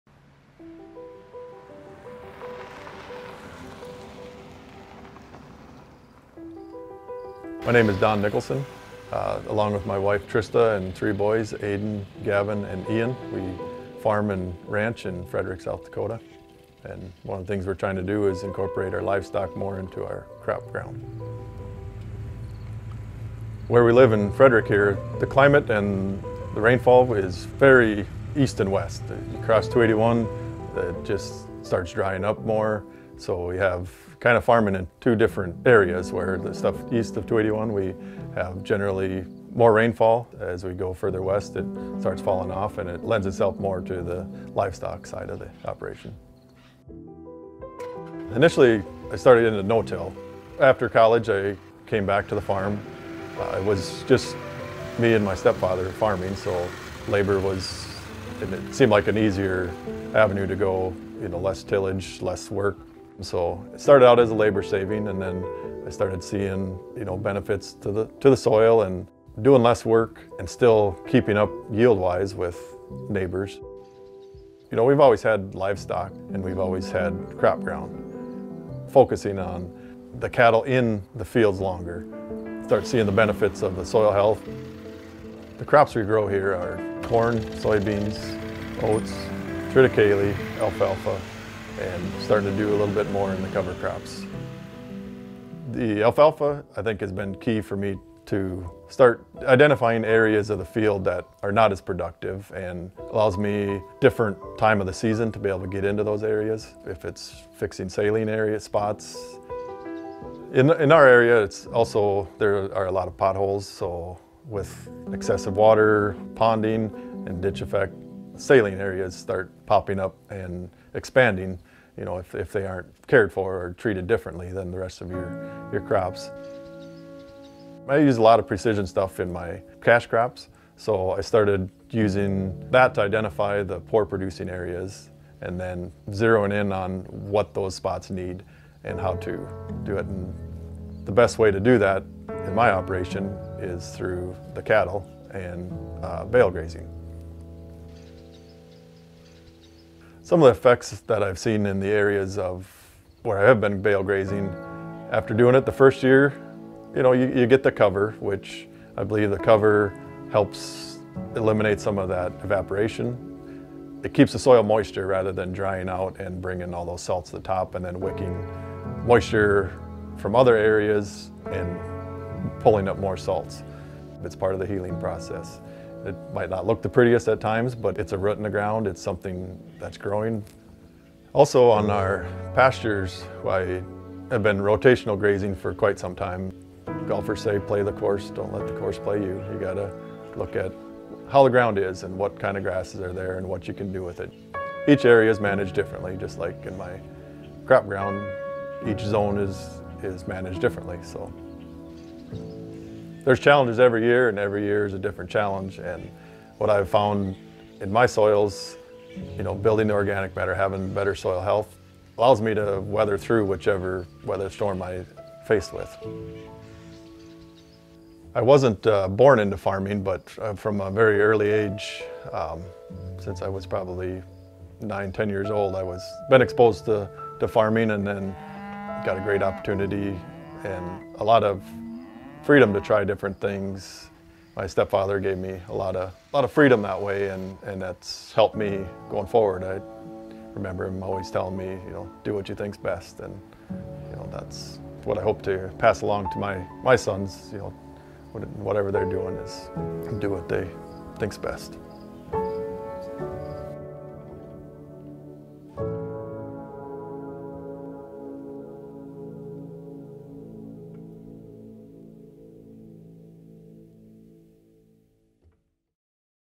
Profiles In Soil Health: "Improving Soil Health & Overcoming Challenges" With Don & Trista Nickelson
Sharing the Success Stories of Agricultural Producers:

-South Dakota’s Conservation Districts
-South Dakota Soil Health Coalition
-United States Department of Agriculture
-Natural Resources Conservation Service